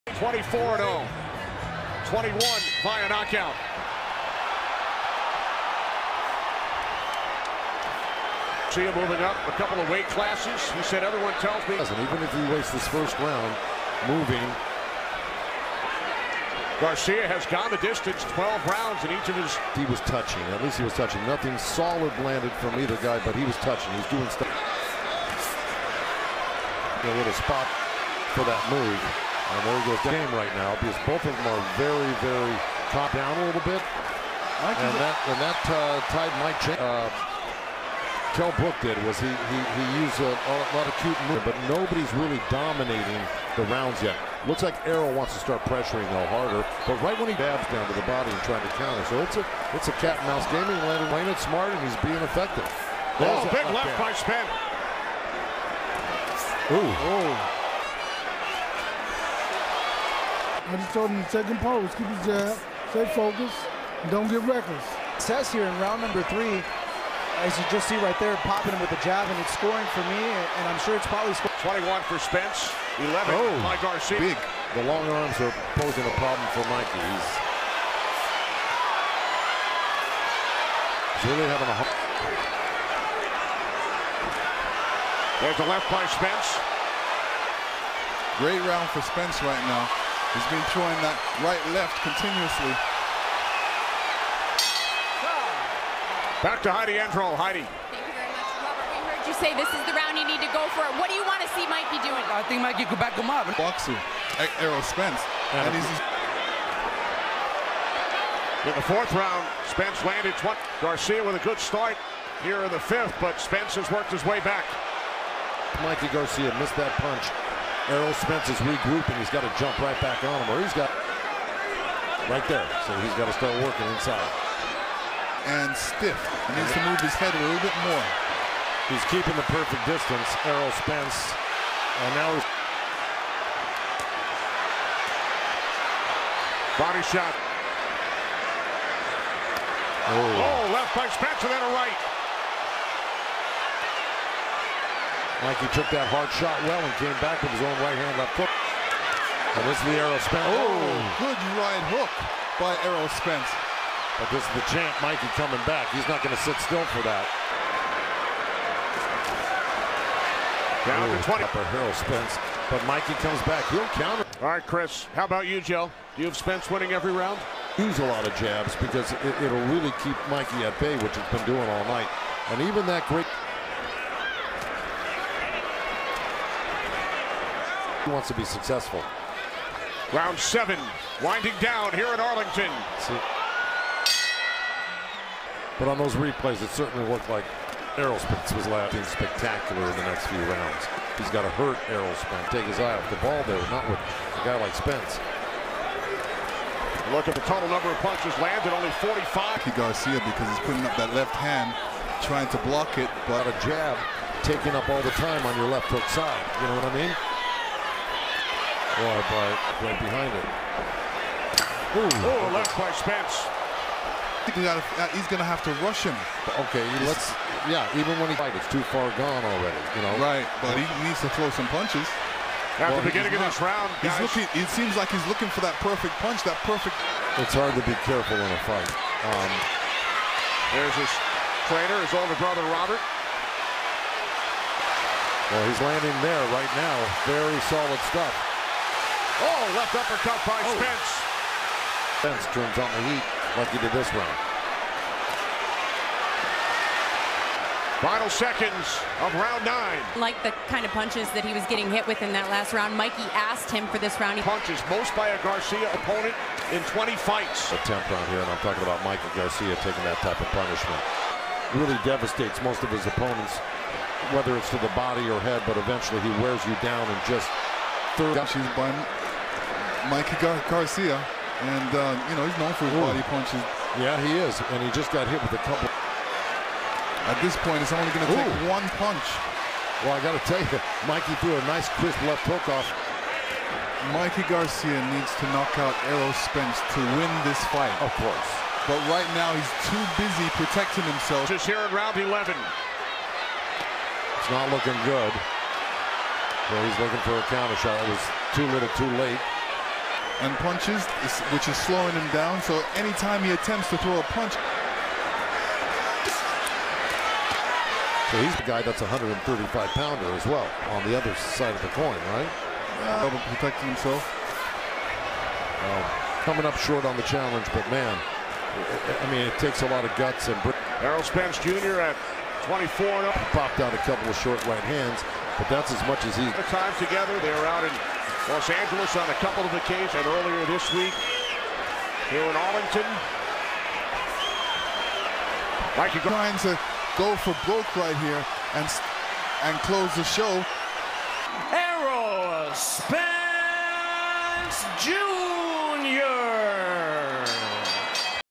Errol Spence Jr Vs Mikey Garcia Highlights (IBF Title)
Errol Spence Jr. Vs Mikey Garcia. Por el Titulo Mundial IBF de Peso Welter. Spence hace la 3ra. Defensa de su Titulo.